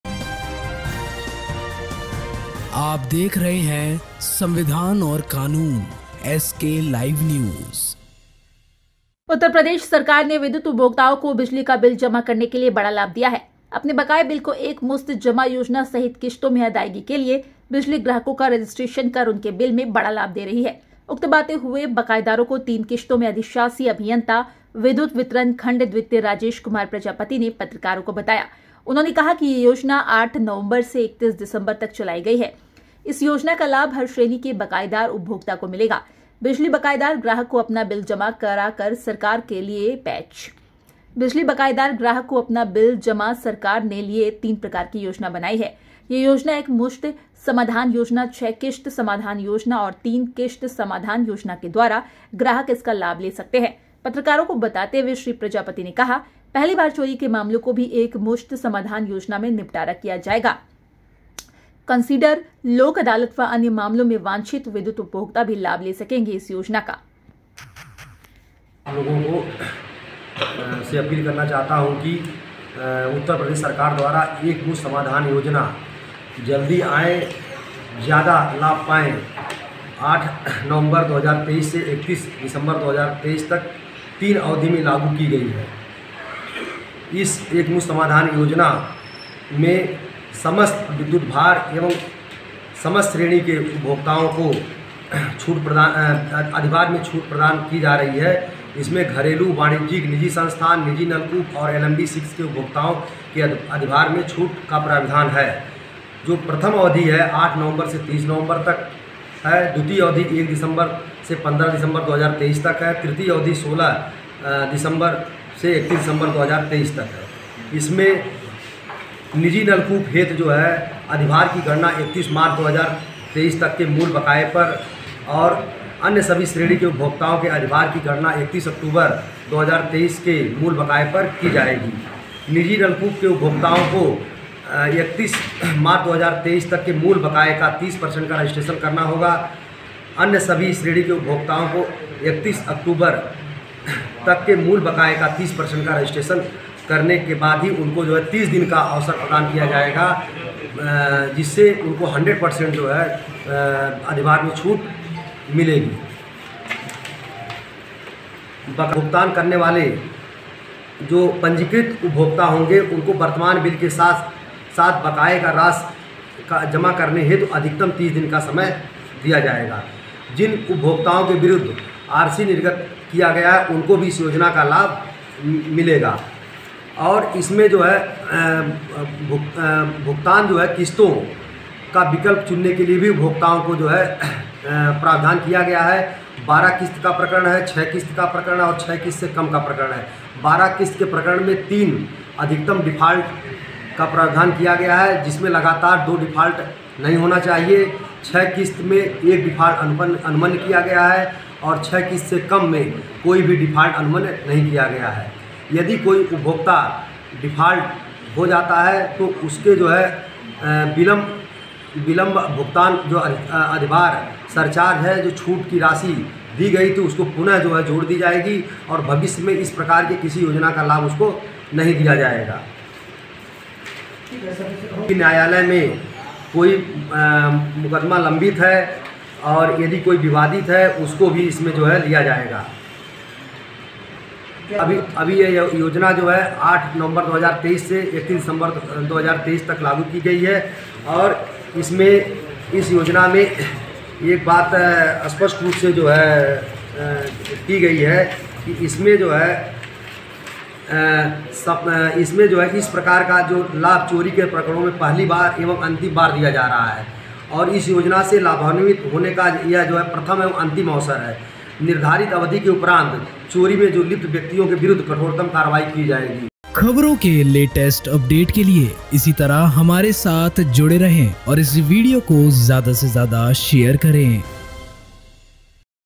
सरकार ने विद्युत बकायेदारों को दिया सौगात
उत्तर प्रदेश सरकार ने विद्युत उपभोक्ताओं को बिजली का बिल जमा करने के लिए बड़ा लाभ दिया है अपने बकाए बिल को एक मुस्त जमा योजना सहित किस्तों में अदायगी के लिए बिजली ग्राहकों का रजिस्ट्रेशन कर उनके बिल में बड़ा लाभ दे रही है। उक्त बाते हुए बकायदारों को तीन किस्तों में अधिशासी अभियंता विद्युत वितरण खंड द्वितीय राजेश कुमार प्रजापति ने पत्रकारों को बताया,उन्होंने कहा की ये योजना 8 नवंबर से 31दिसंबर तक चलाई गई है इस योजना का लाभ हर श्रेणी के बकायेदार उपफोक्ता को मिलेगा, बिजली बकायेदार ग्राहक को अपना बिल जमा  सरकार ने लिए तीन प्रकार की योजना बनाई है।यह योजना  एक मुस्त समाधान योजना छ किस्त समाधान योजना और तीन किस्त समधन योजना के द्वारा ग्राहक इसका लाभ ले सकता है। पत्रकारो को बताते हुए श्री प्रजापति ने कहा की पहली बार चोरी के मामलों को भी एक मुस्त समाधान योजना में निपटारा किया जाएगा कंसीडर, लोक अदालत व अन्य मामलों में वांछित विद्युत उपभोक्ता भी लाभ ले सकेंगे इस योजना का लाभ